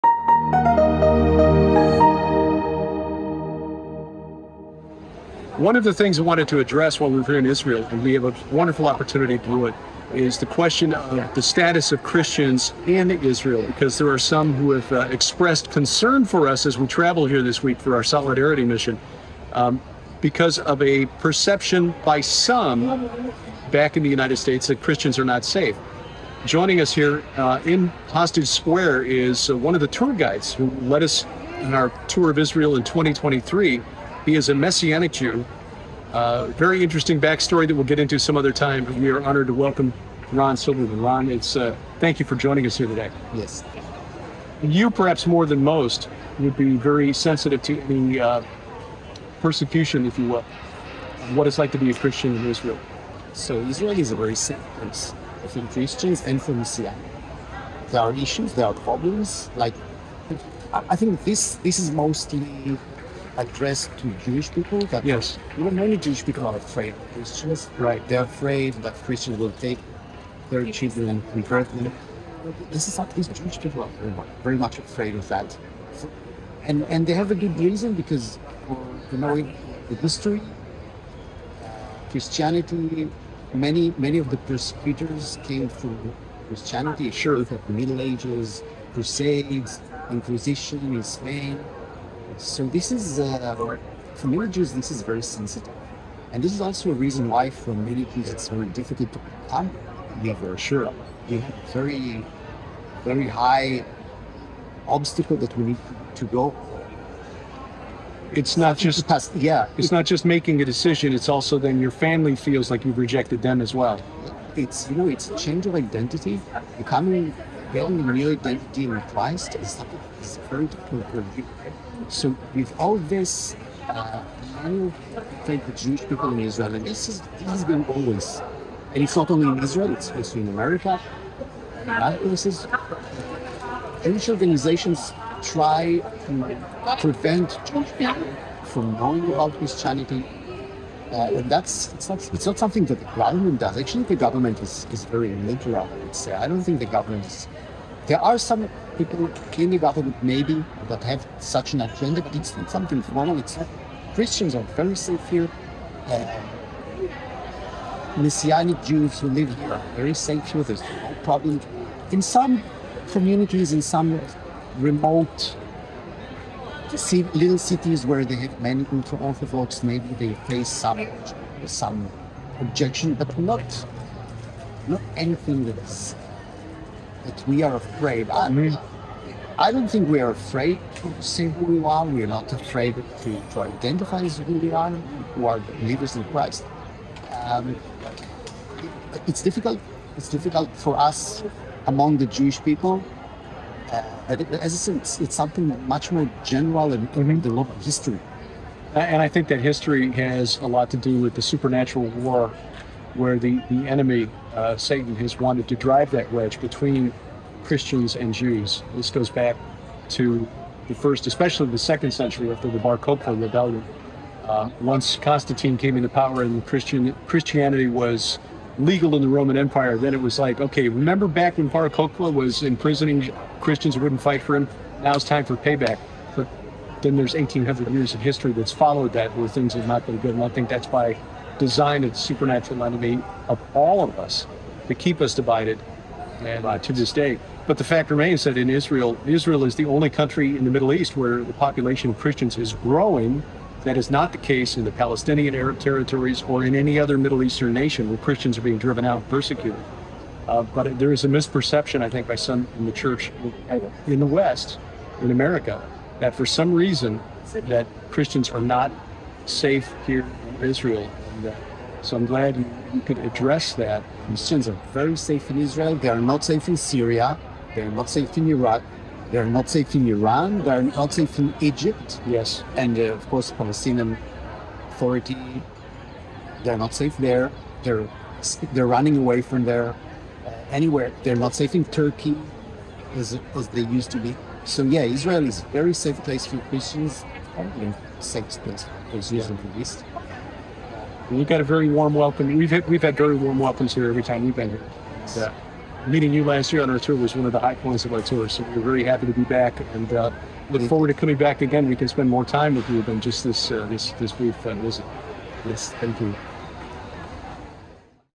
Christians Are Welcome in Israel
There are some in the West who believe Israel persecutes Christians. Israeli Ran Silberman, a messianic Jew, says that's just not true.